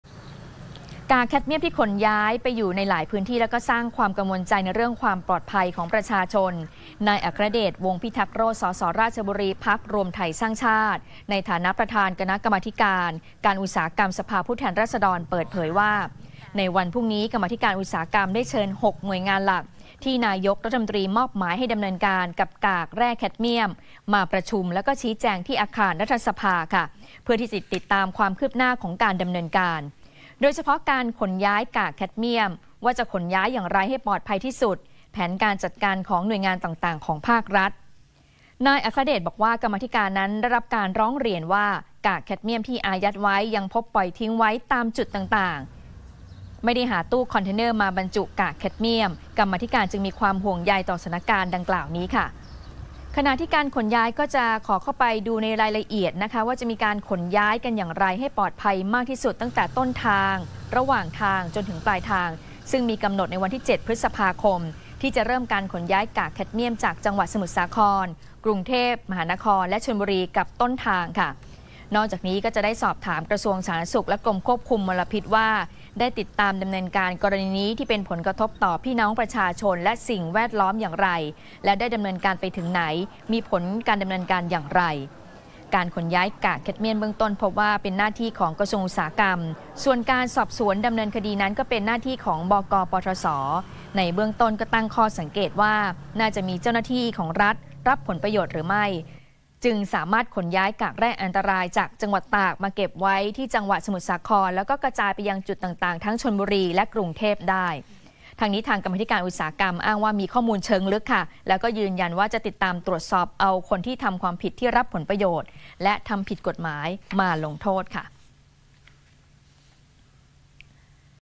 กมธ.อุตสาหกรรม เรียกแจงปมขนย้าย "แคดเมียม" l ทันข่าว l 16 เม.ย. 67
วันพรุ่งนี้ (17 เม.ย. 67) กมธ.อุตสาหกรรม เรียก 6 หน่วยงาน ชี้แจง ปมขนย้ายกากแคดเมียม ย้ำต้องอยู่ในพื้นฐานความปลอดภัยสูงสุด หากมีเจ้าหน้าที่รัฐรับผลประโยชน์ จะนำตัวมาลงโทษ